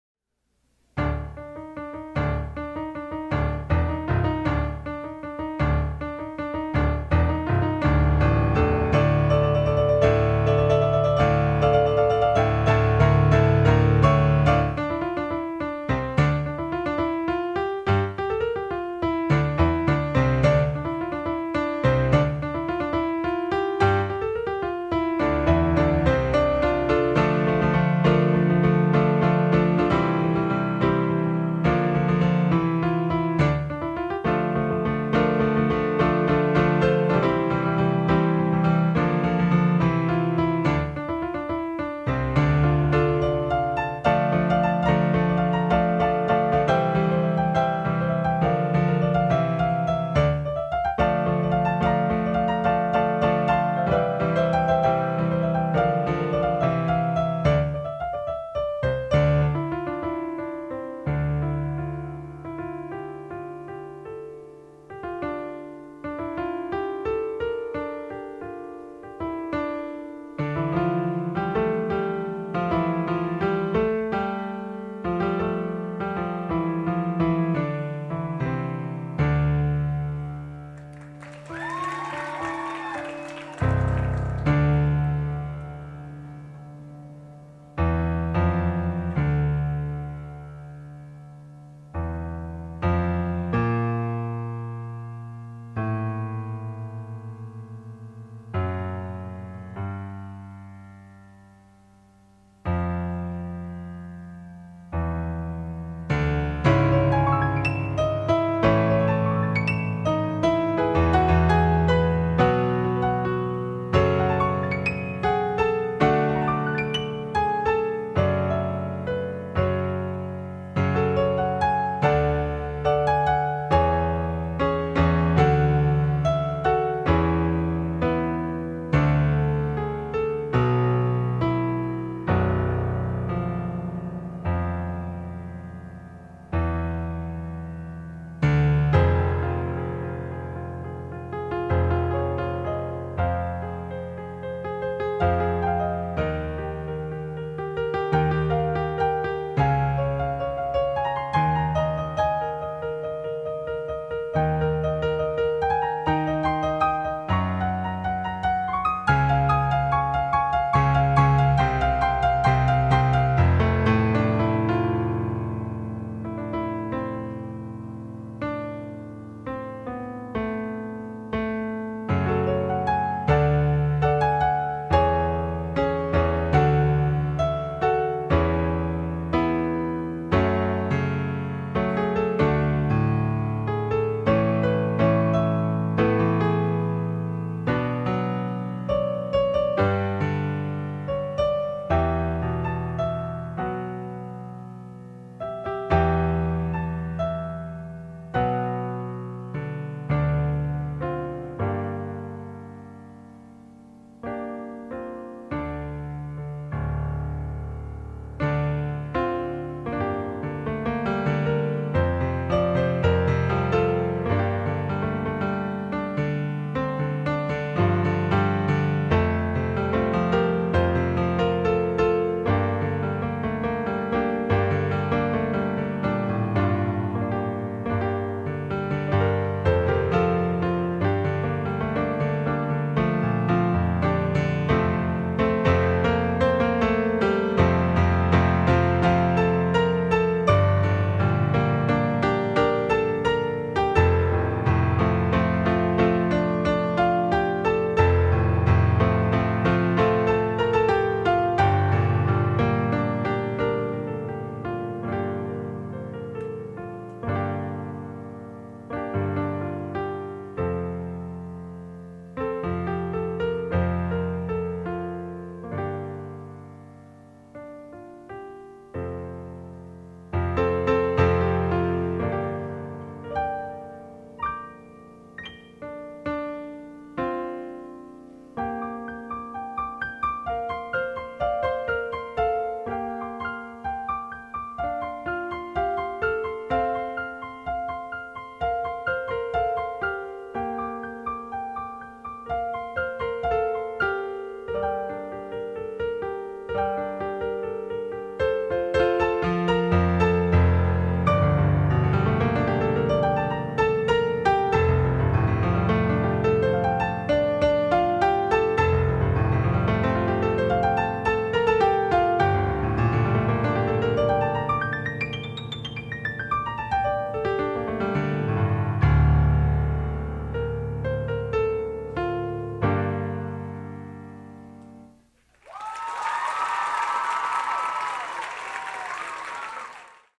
HRHS TNT 2017 - 8 Piano Medley
The Heritage Regional High School TNT Secondary 5 Talent Show. Thursday April 20, 2017.